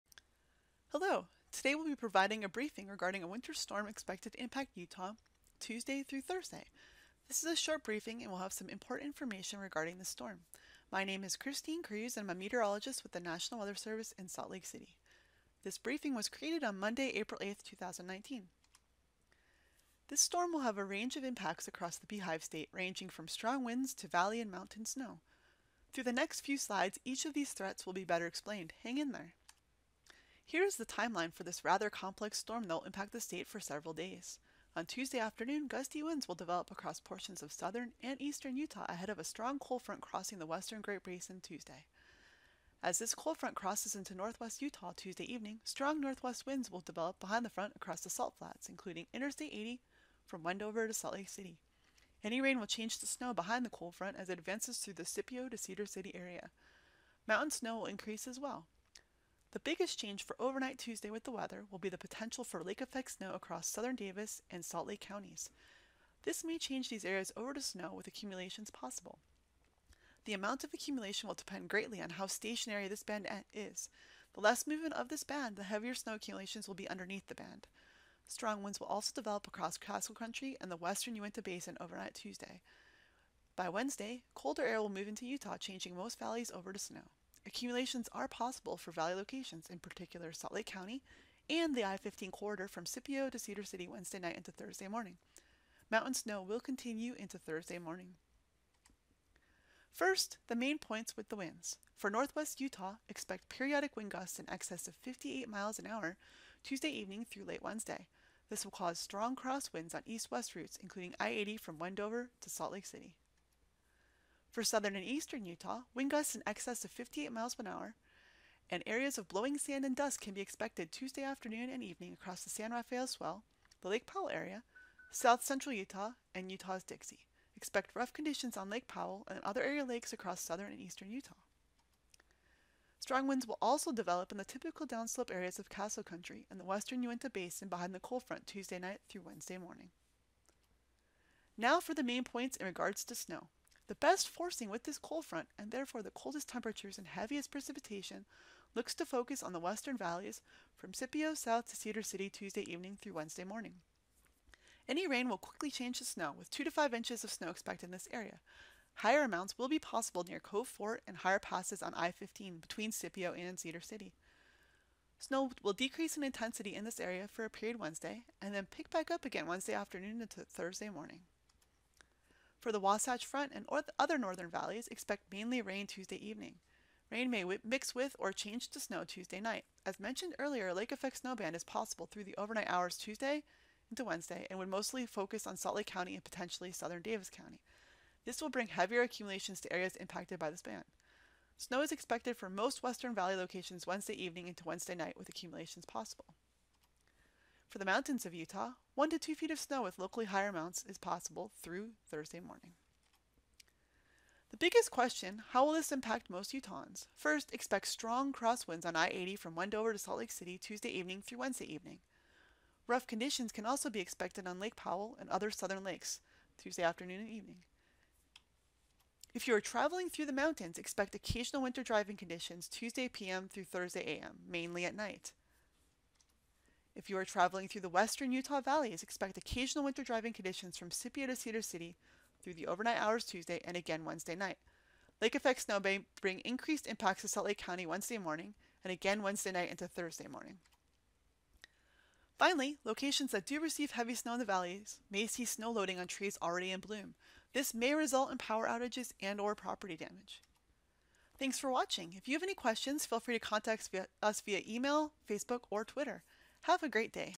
Winter Storm Briefing April 8, 2019
A late season winter storm will impact Utah Tuesday through Thursday bringing wide ranging impact to the state.